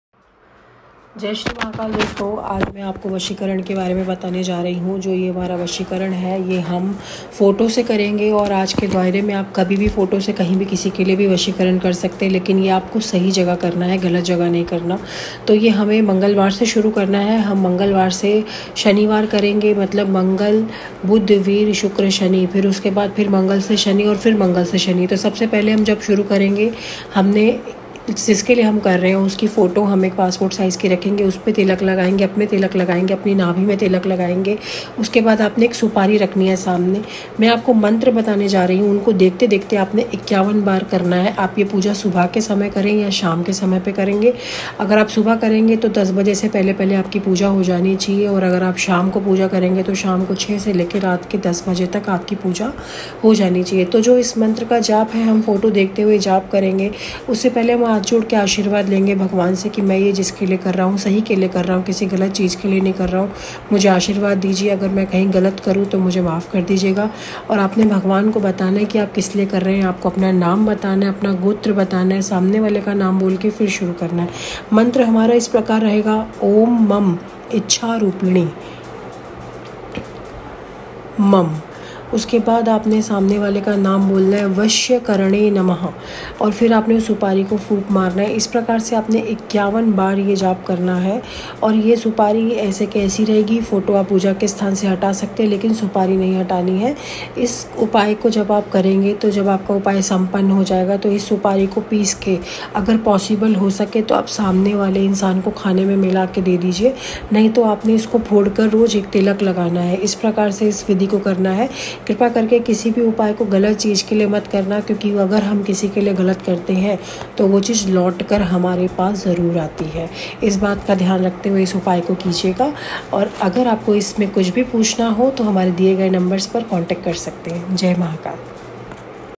जलदी से ओर सरल तरीक़े से करे फ़ोटो से वशीकरण
कई लोगों का ऐसा मानना है कि Mobile photo se vashikaran करना संभव नहीं है। पर यह एकदम गलत बात है मोबाइल के फोटो से भी वशीकरण को किया जा सकता है। आज हम आपको जो विधि बताने जा रहे हैं उस विधि का उपयोग करके आप अपने मोबाइल के फोटो से ही इच्छित व्यक्ति या महिला का वशीकरण कर सकते हैं।